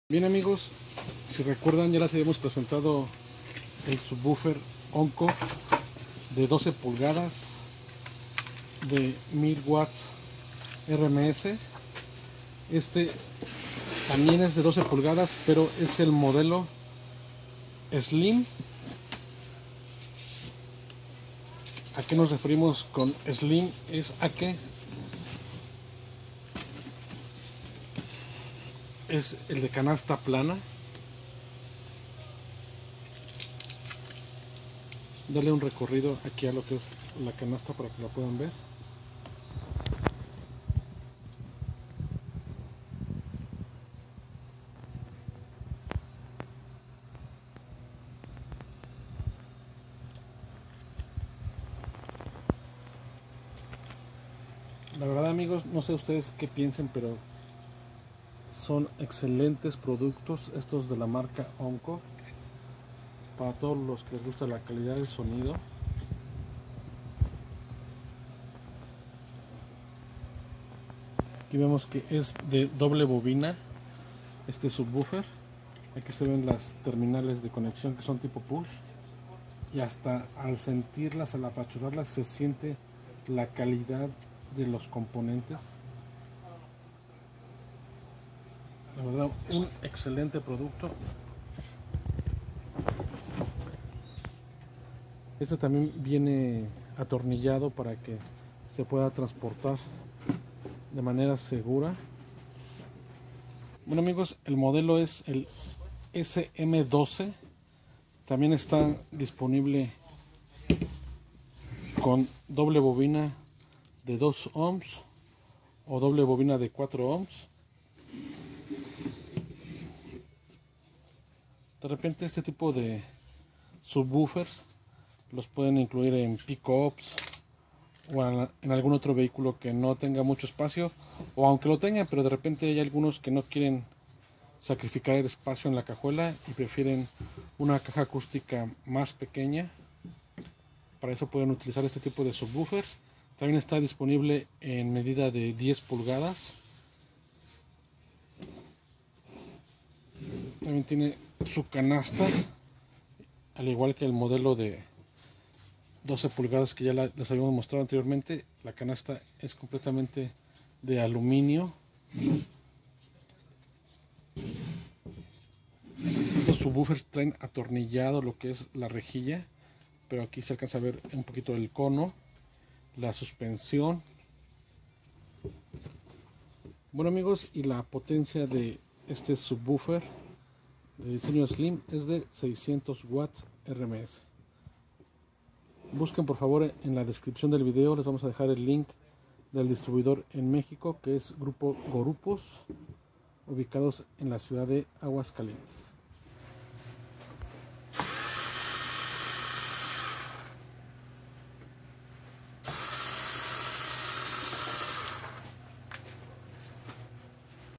¡Subwoofer SLIM para Calidad de Sonido! 600 Watts / Marca Oncore
Si te gusta la Calidad de Sonido y buscas un subwoofer diferente a lo que actualmente hay en el mercado de Car Audio, te invito a probar este subwoofer de 12 pulgadas tipo Slim de la marca Oncore, maneja una potencia de 600 Watts RMS.
Comparto el link del Facebook del distribuidor en México de Oncore: SM12